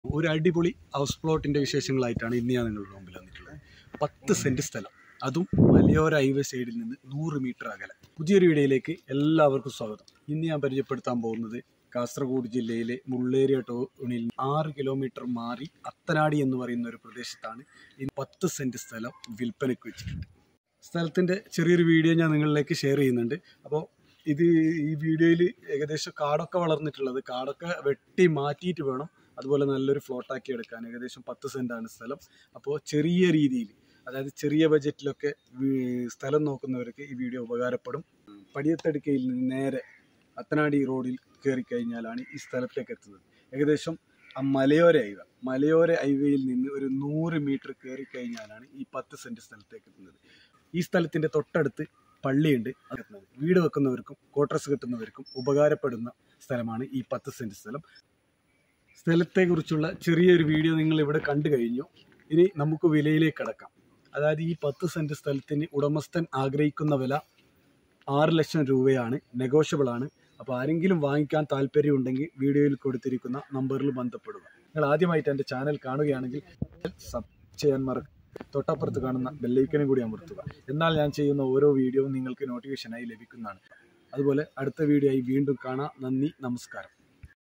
house plot for sale
*ഉടൻ വിൽപനക്ക്*
*കാസറഗോഡ് ജില്ലയിലെ മുള്ളേരിയ ട്ടൗണിൽ നിന്ന് 6 കീ മീ മാറി അത്തനാടി എന്ന പ്രദേശത്ത്   ഒരു  10 സെൻ്റ് ഹൗസ്പ്ലോട്ട് വിൽപനക്ക്*

*✅ NB: പ്രോപ്പർട്ടി വിൽക്കുന്നതിനും വാങ്ങുന്നതിനും, YouTube Channel ൽ പരസ്യം നൽകുന്നതിനും വിളിക്കാം. *